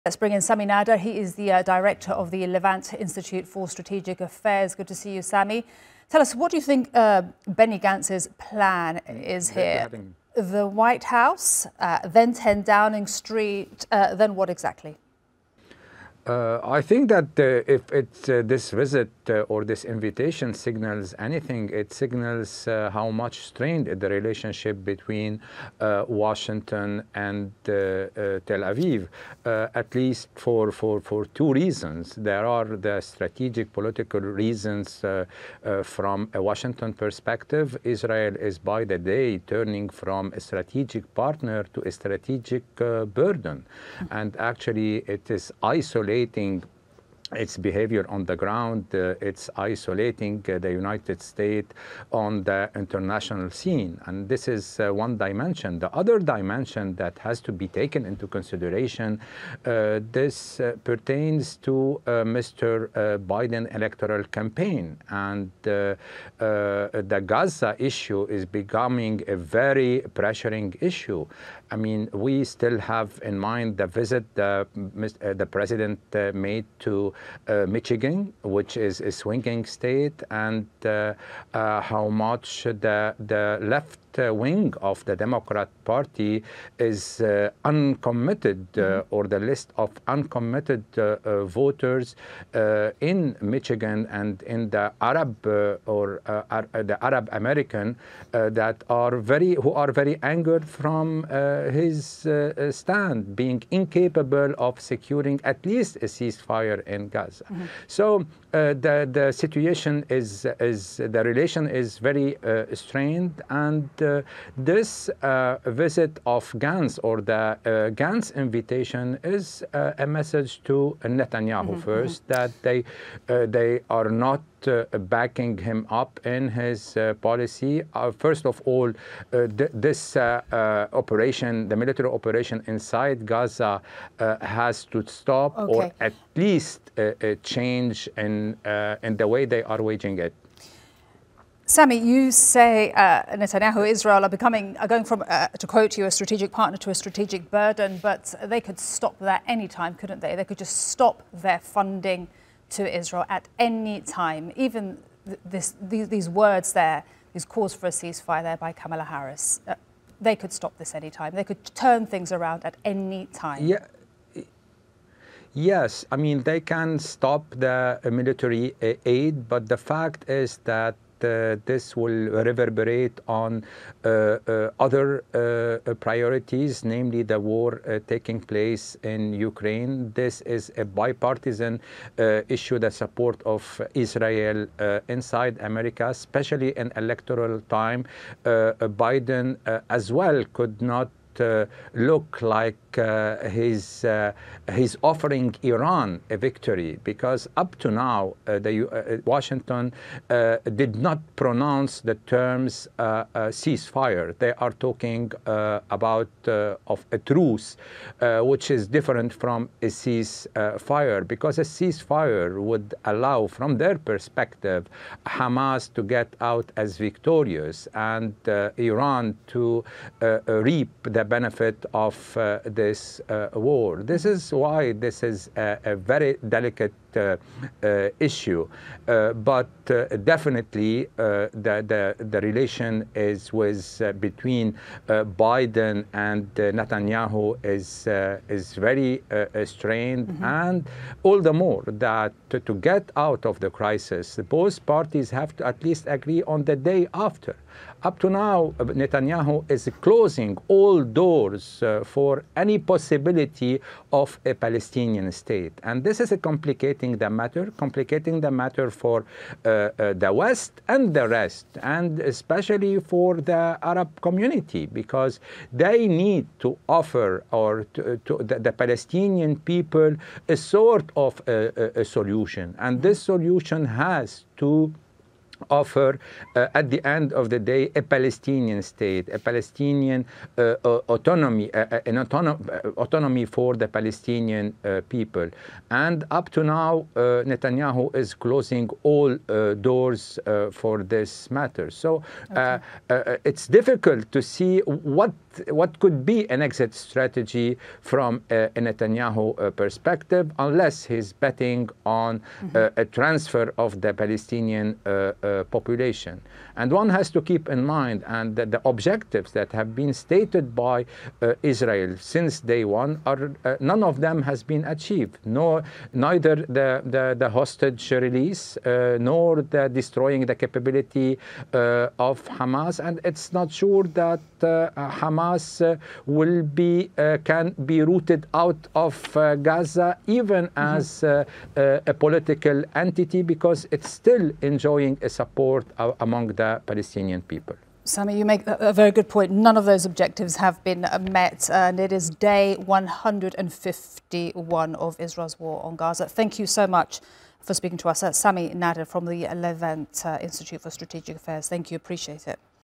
What does Benny Gantz’s visit to the US signal?
War cabinet minister Benny Gantz has travelled to Washington for talks as cracks show in the Israeli leadership. Sami Nader from the Levant Institute for Strategic Affairs discusses the significance of this visit.